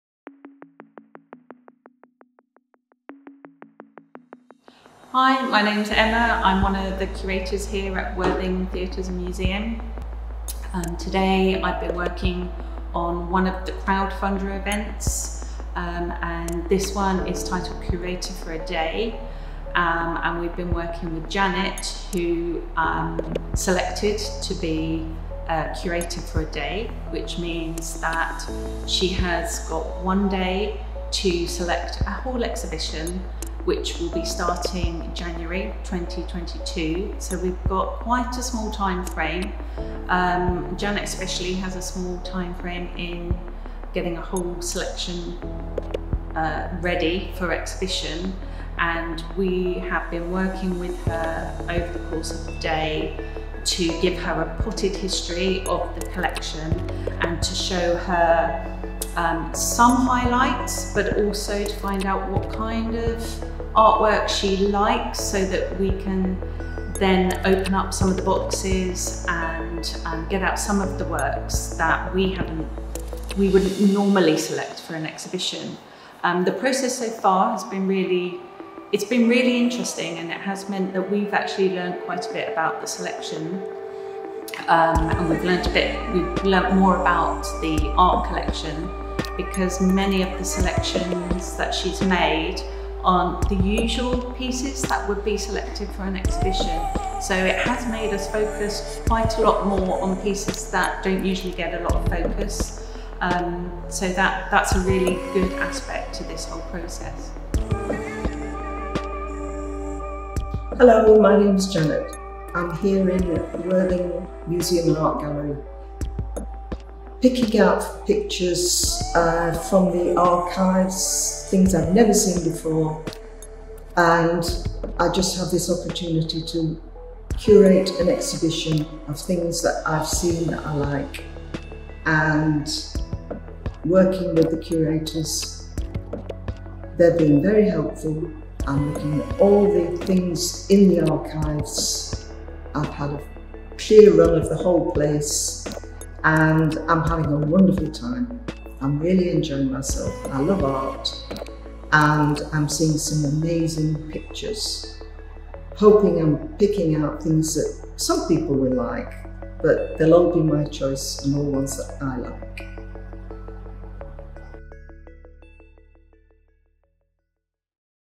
Curator for a Day Exhibition
To help raise much needed funds during the pandemic and subsequent temporary closure of the Museum and Art Gallery, an extra special ‘reward’ was made available through our Crowdfunding campaign; one member of the public would become a ‘Curator for a Day’, and select their favourite works of art from amongst our permanent collection to go on display in the Norwood Gallery in their very own exhibition!

This lucky person was Janet; an art enthusiast who had always been interested in the behind-the-scenes aspects of curating exhibitions, and who was thrilled to have a chance to have a go herself.  Along with museum staff, Janet spent two half days looking through the art store, choosing from amongst the huge range of paintings, prints and drawings in the collection.

Curator of Art and Exhibitions, Emma Walder, and Assistant Curator, Sarah Hobson, provided Janet with some important information to help her make her choices. They talked safely handling the various types of artistic media and their individual storage and display requirements, as well as discussing both the highlights of the art collection and the lesser displayed works too. Janet was drawn to so many of the fantastic examples in our collection, that whittling down the longlist of favourites was perhaps the biggest challenge of all.

This exhibition brings together Janet’s final choices and some insight into how and why she made her selections.

The exhibition will run in WTM's Norwood Gallery from Saturday 15 January 2022 - Sunday 3 April 2022.